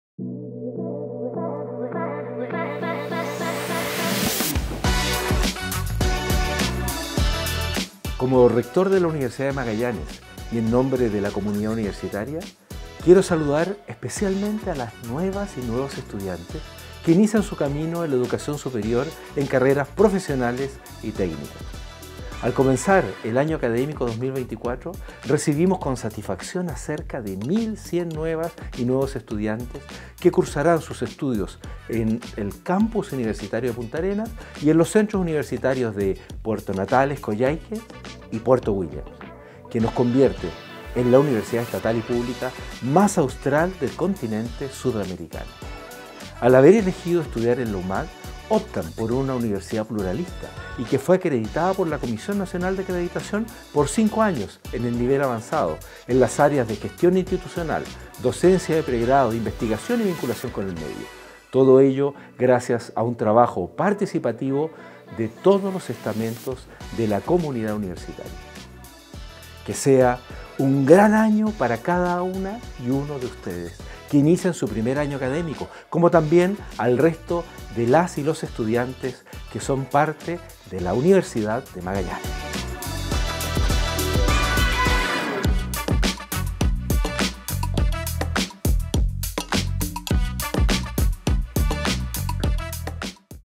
Rector José Maripani saluda a nuevas y nuevos estudiantes de la UMAG
El rector José Maripani comparte un afectuoso saludo a las nuevas y nuevos estudiantes de la Universidad de Magallanes, con los mejores deseos para el año académico que se inicia.